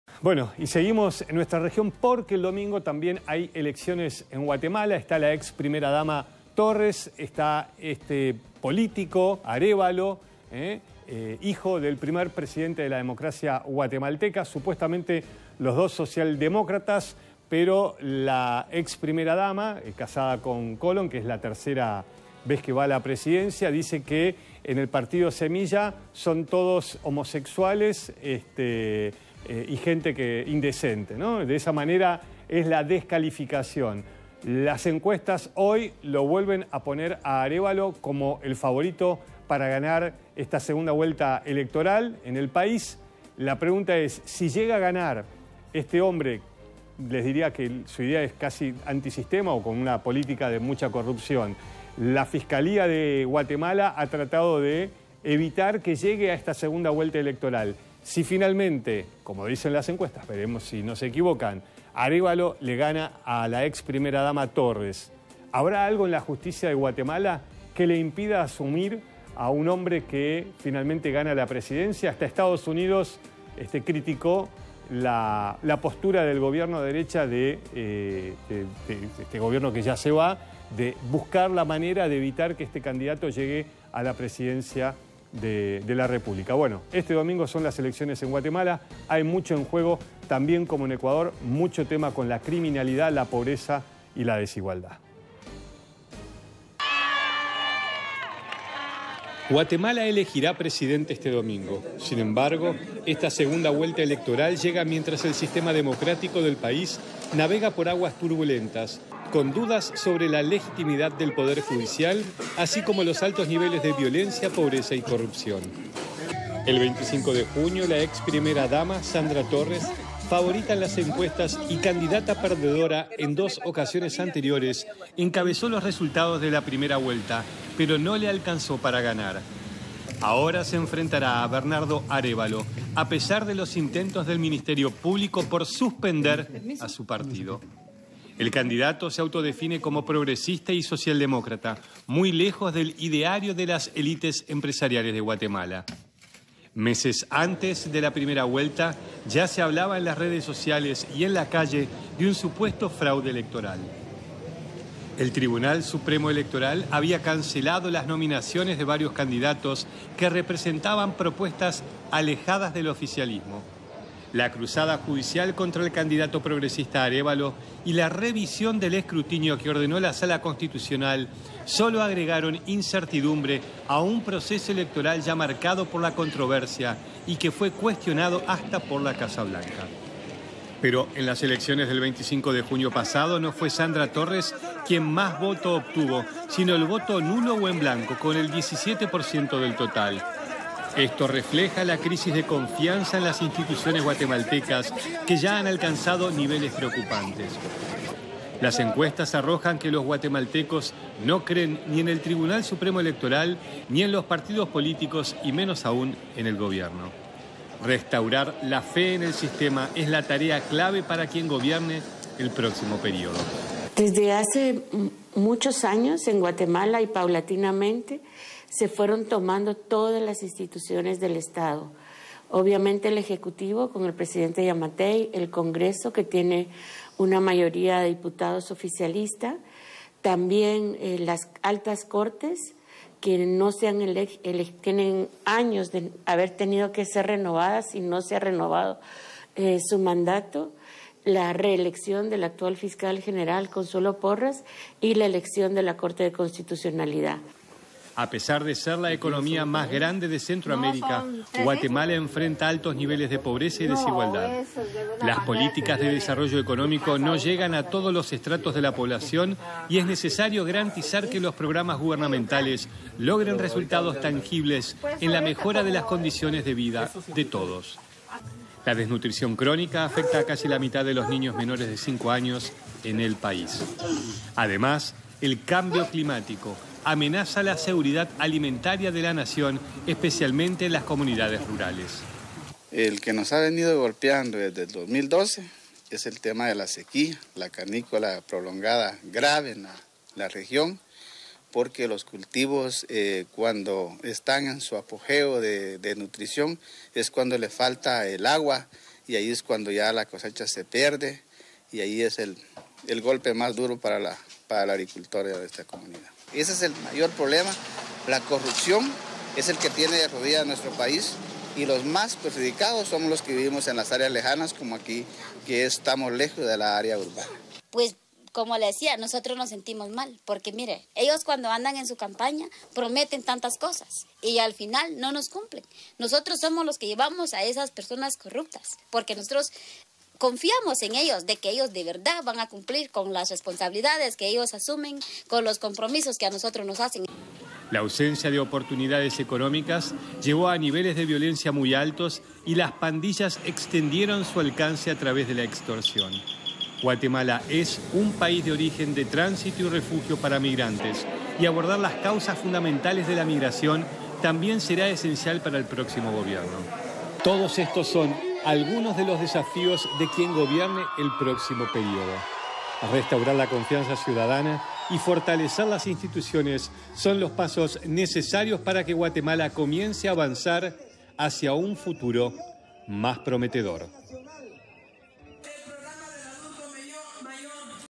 Entre polémicas y denuncias, Guatemala elige presidente
👉 La segunda vuelta electoral incluyó la revisión de actas tras la primera vuelta y una investigación abierta por posibles irregularidades en la inscripción del candidato Bernardo Arévalo del partido Semilla.